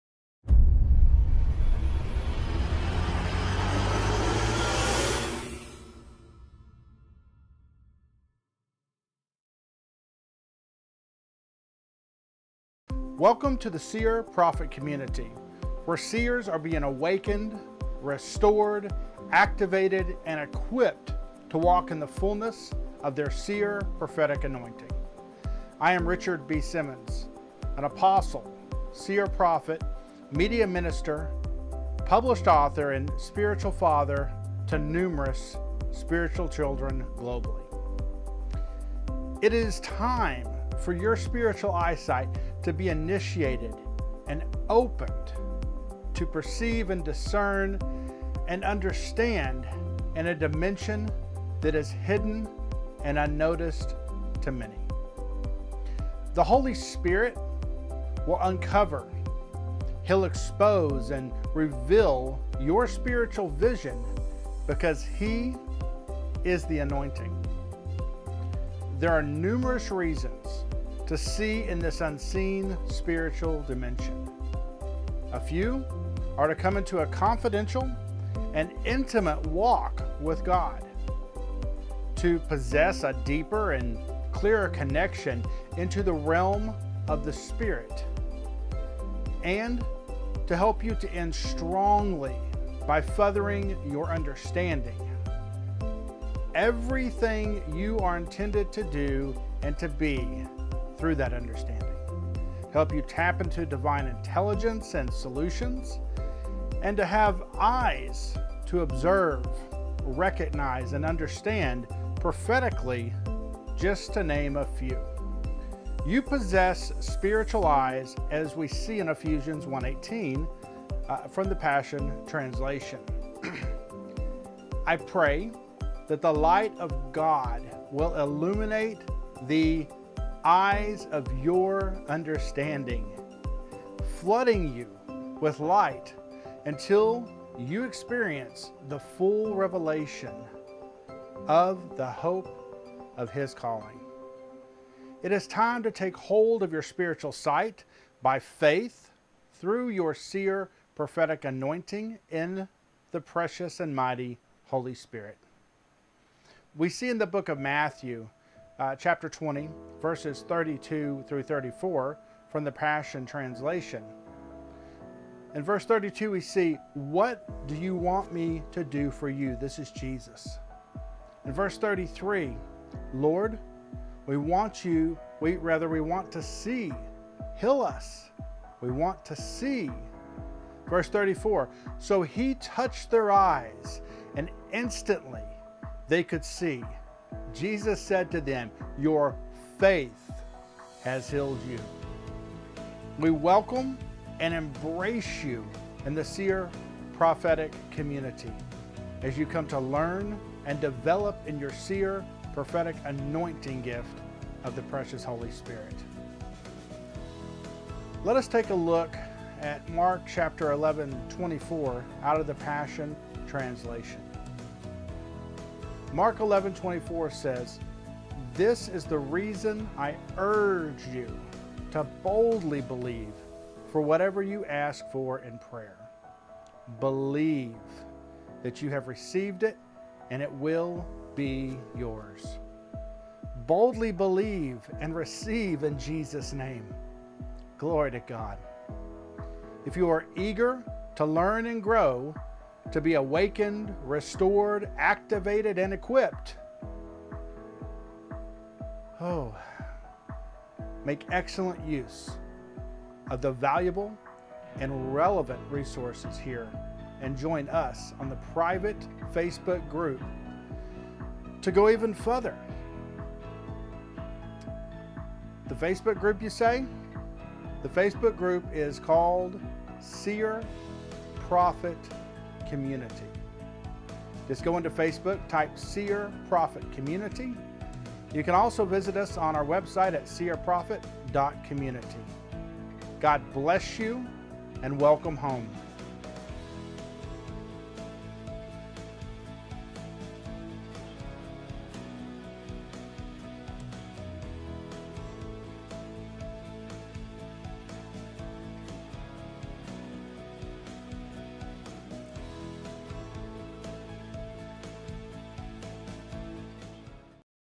Welcome To The Seer Prophet Community
Welcome To The Seer Prophet Community! A welcome from Apostle and Seer prophet Richard B Simmons.